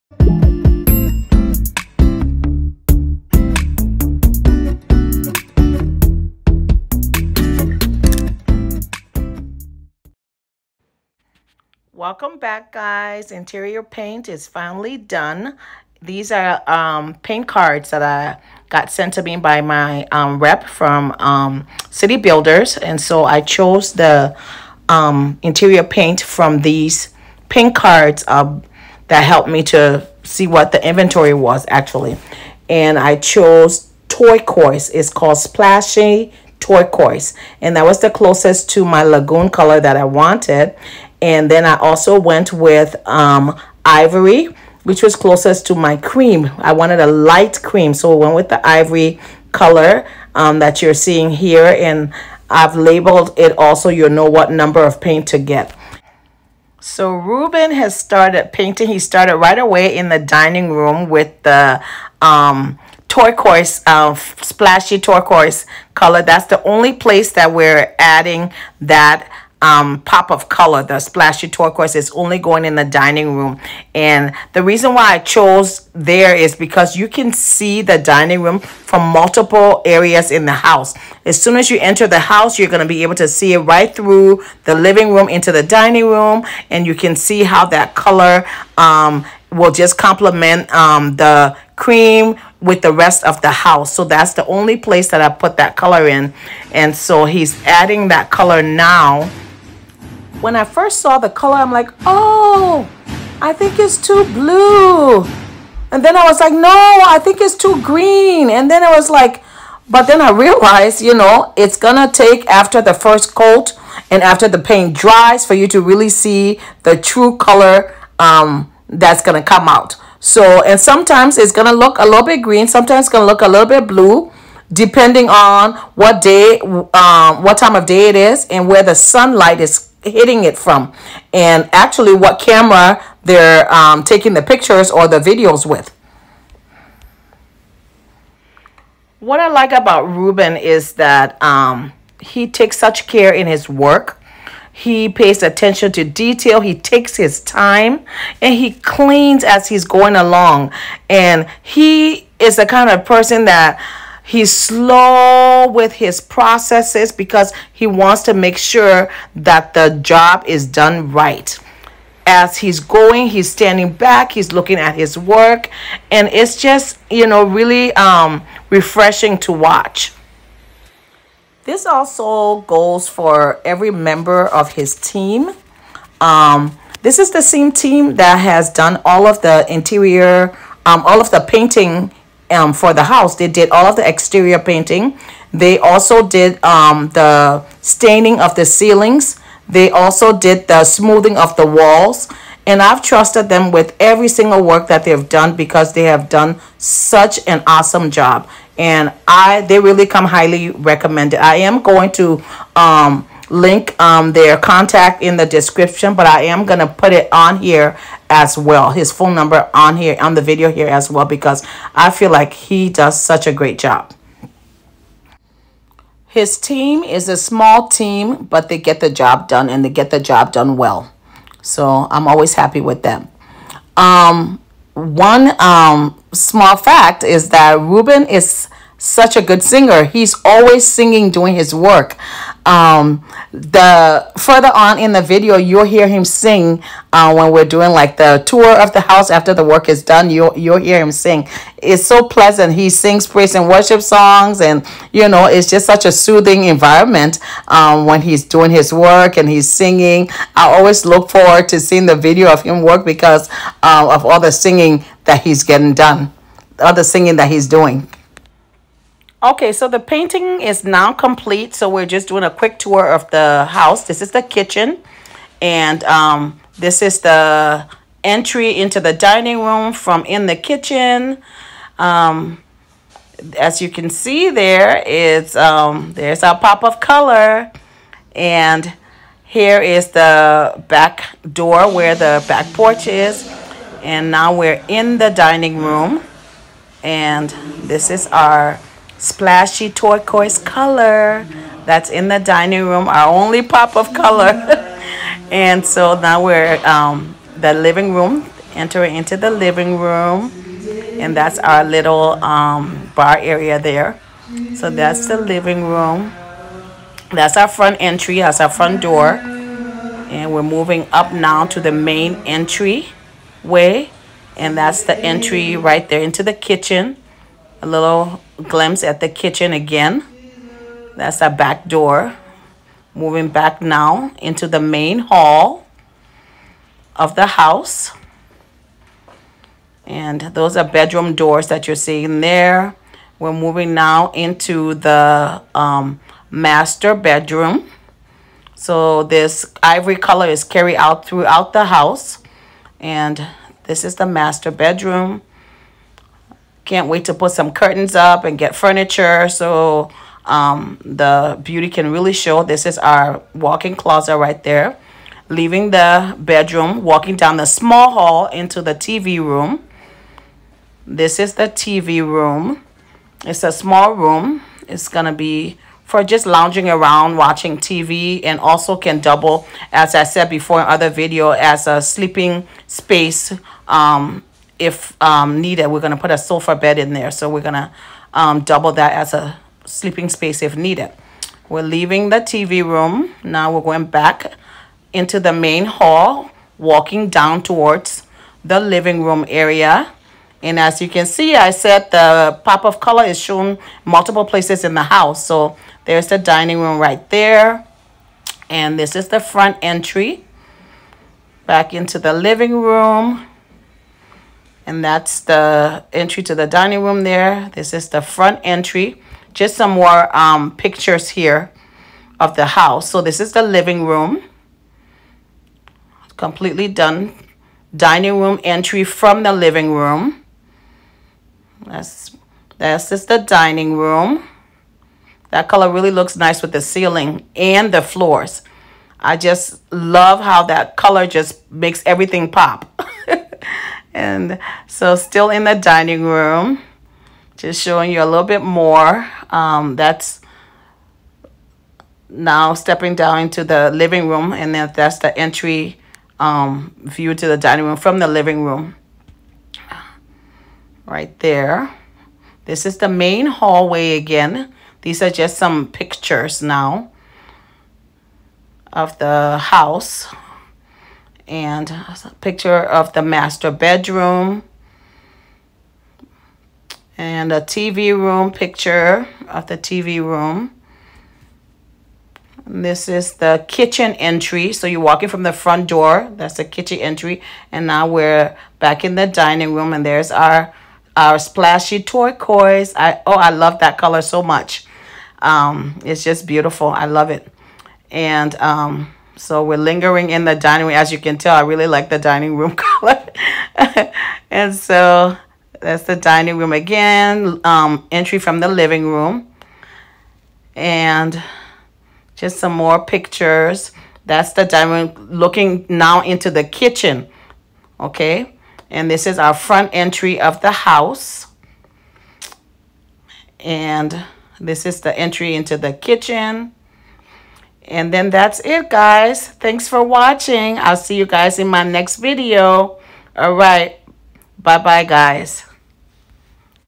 Building My House In Liberia, West Africa 🇱🇷 2021 (Episode # 29) Final Interior Paint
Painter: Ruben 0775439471